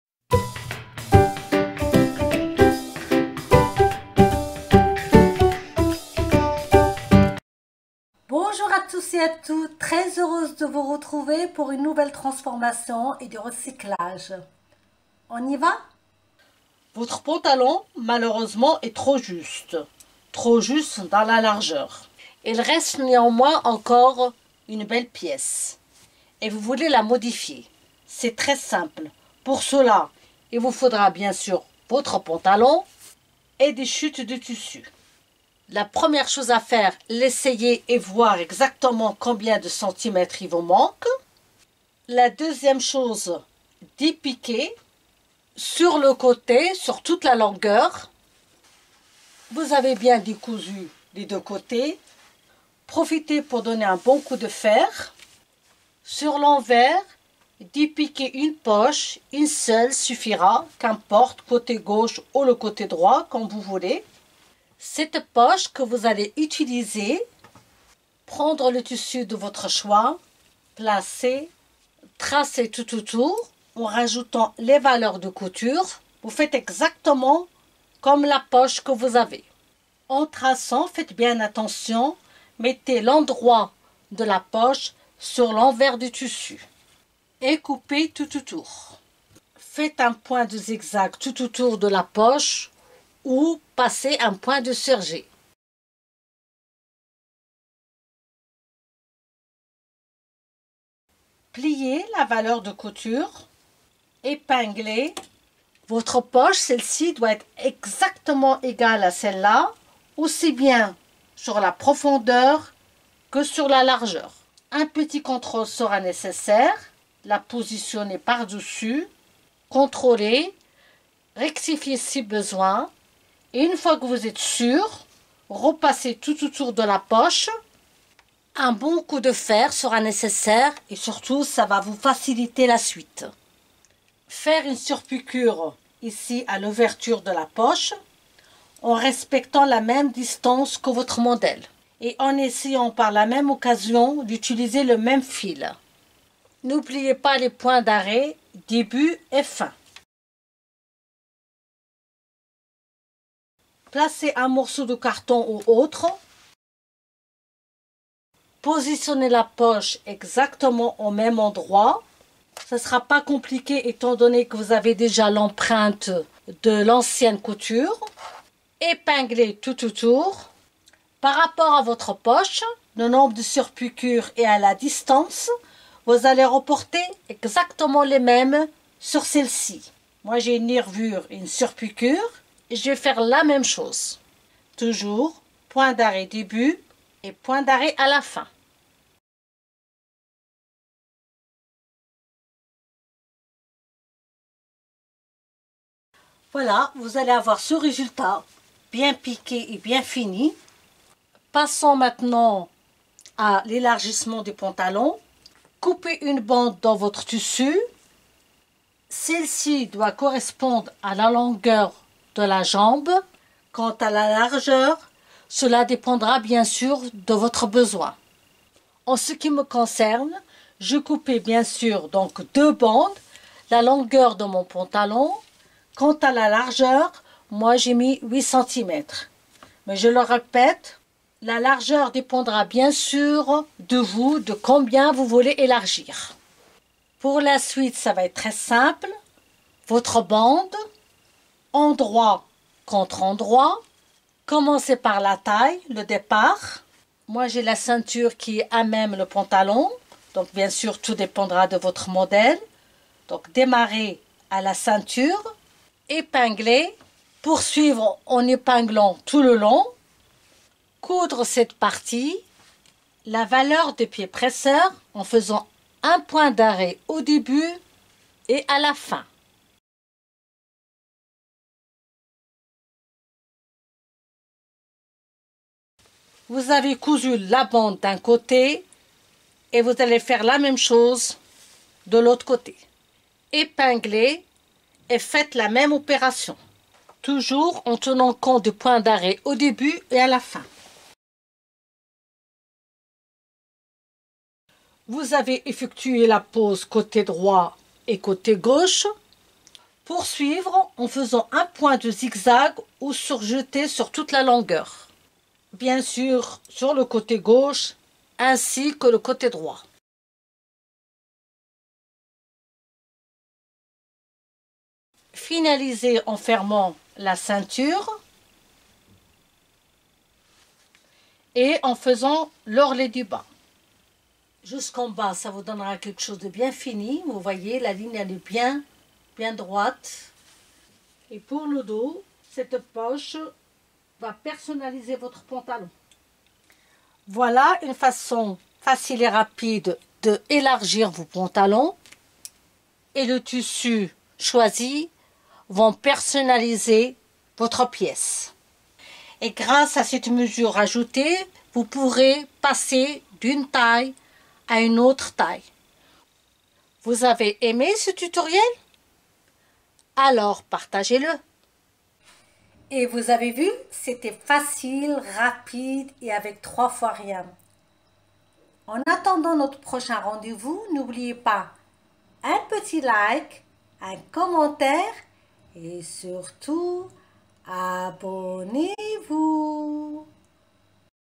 DIY # 144 Comment élargir le pantalon .Mode tendance 2021. + le personnaliser . Facile et rapide
Comment élargir son  pantalon. Le pantalon a rétrécie ou simplement vous avez pris une taille de plus , ou  on vous a donner un ,mais malheureusement il est trop juste pour vous .
Qu'importe la raison ...La mode cette année est le pantalon large .
A travers ce tutoriel vous allez apprendre à remettre le pantalon à vos mesures .
Une chute de tissu et de la patience et vous voilà reparti avec un modèle stylisé et personnalisé .
Alors redonner une seconde chance à vous vêtements et surtout ne jetez plus .
Bonne réalisation à tous

How to widen his pants. The pants have shrunk or simply you have taken one more size , or you have been given one ,but unfortunately it is too fair for you .
Whatever the reason ...
Through this tutorial you will learn how to put the pants back to your measurements .
A drop of fabric and patience and here you go again with a stylized and personalized model .
So give back a second chance to your clothes and especially do not throw anymore.
Achievement for all